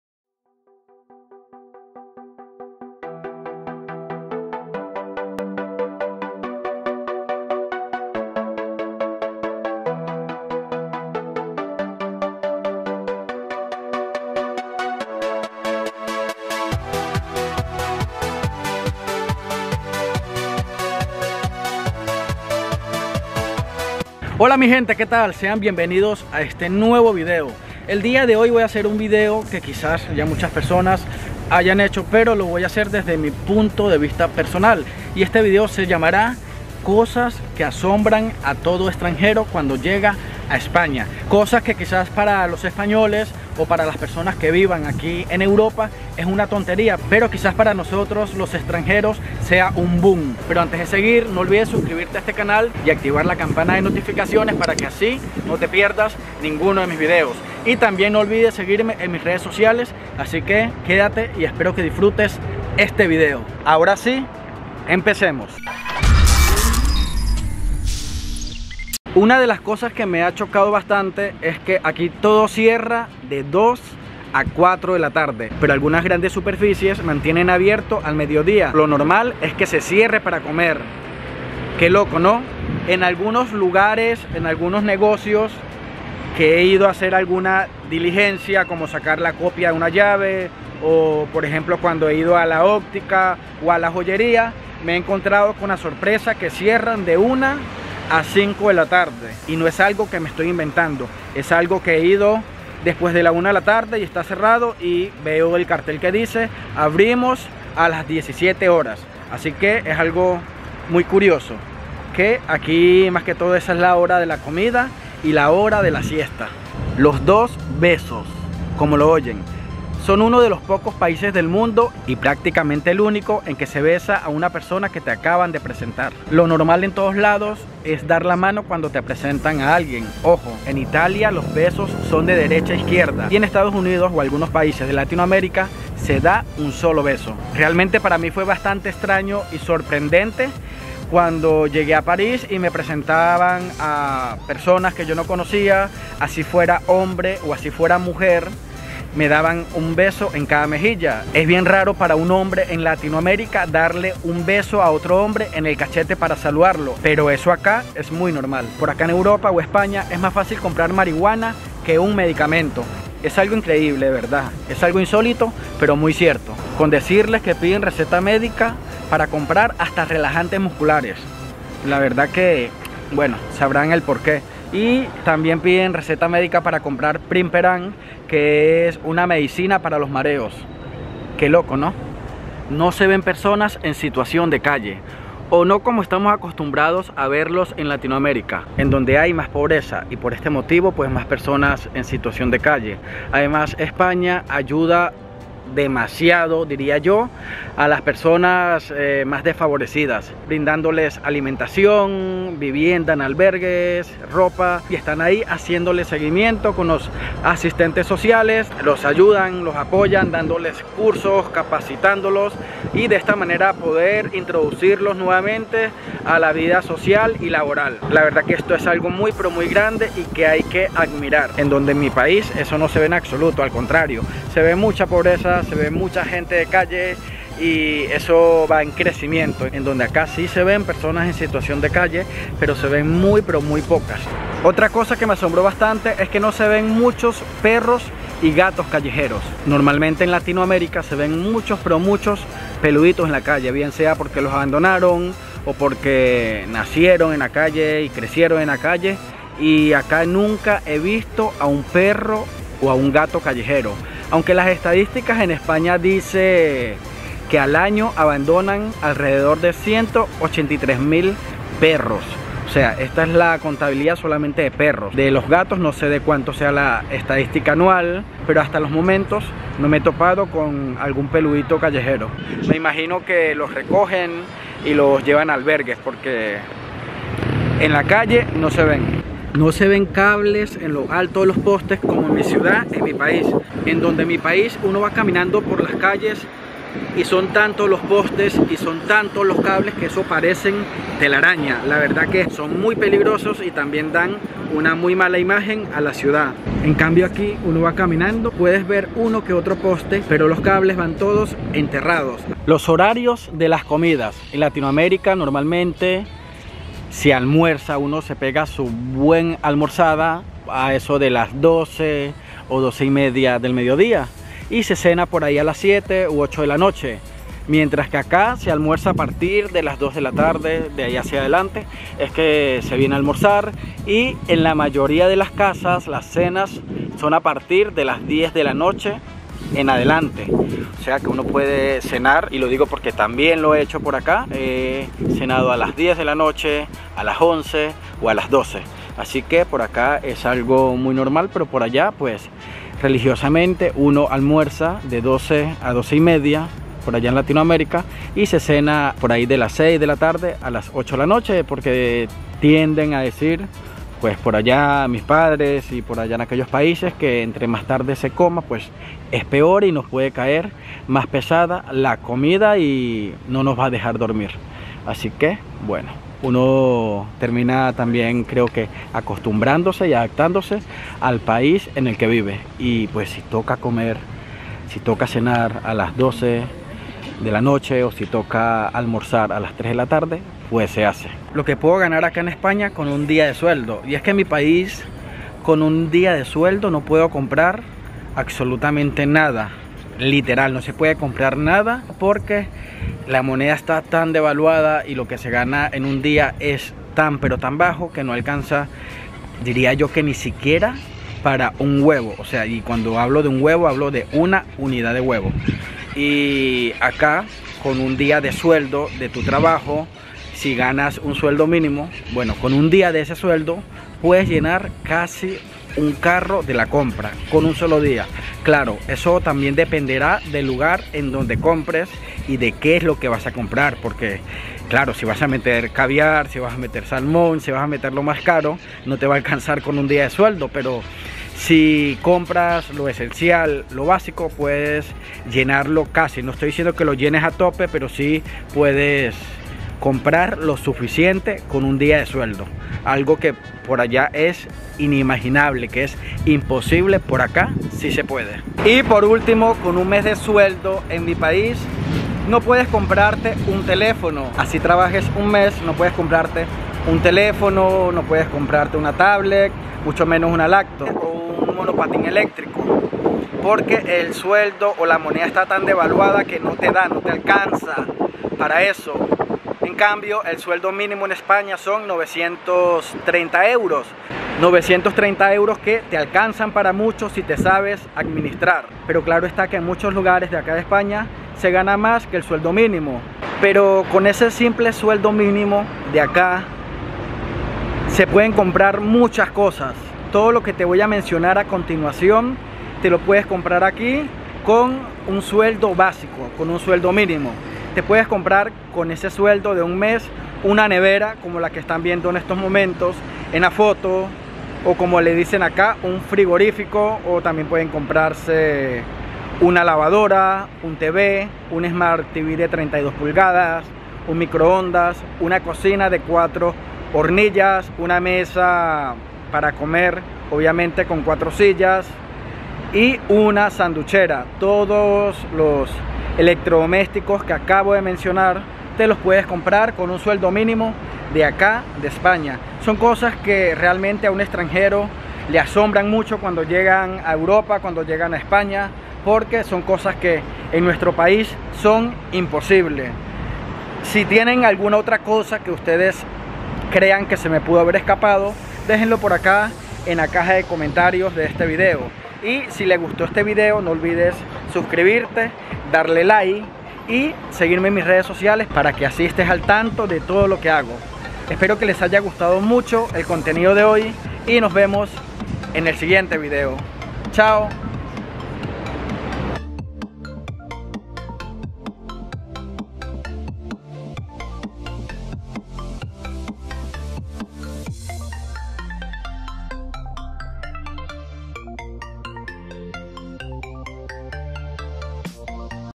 COSAS QUE SORPRENDEN DE ESPAÑA
Hey mis amigos por acá les dejo mi ultimo video ,en donde les menciono algunas cosas que me sorprendieron al llegar a España, ya con el tiempo terminas acostumbrándote, espero les guste y espero contar con tu apoyo gracias .
apoya el canal aca ,muchas gracias.